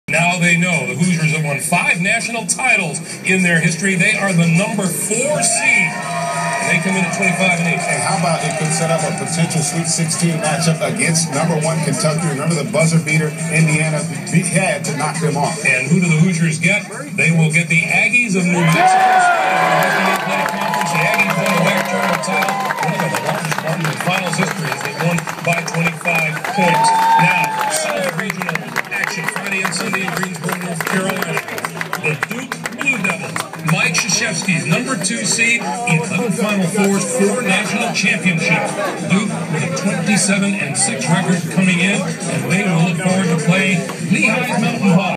NMSU Aggies watch party
NMSU Aggie basketball team finds out where and whom they will play in the 2012 NCAA Tournament